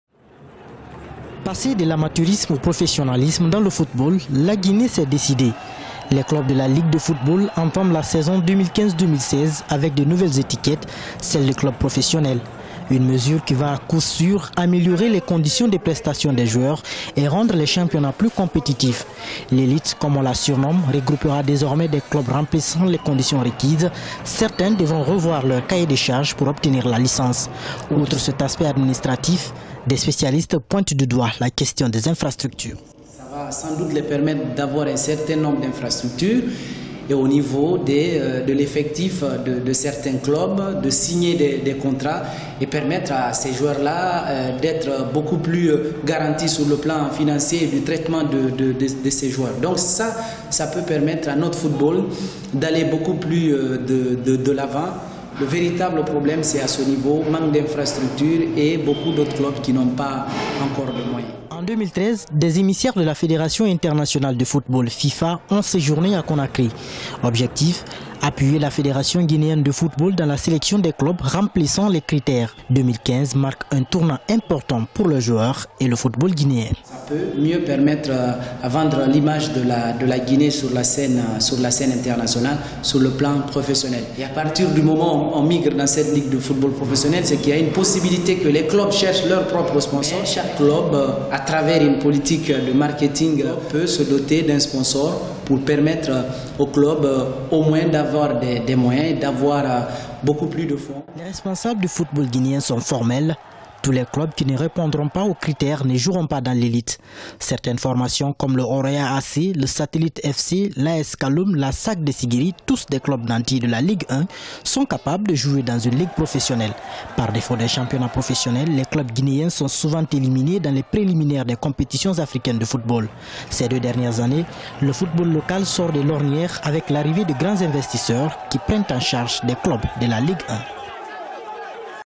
Guinée, La Ligue de football opte pour la professionnalisation
Sport, GUINEE: vers la professionnalisation de la Ligue de football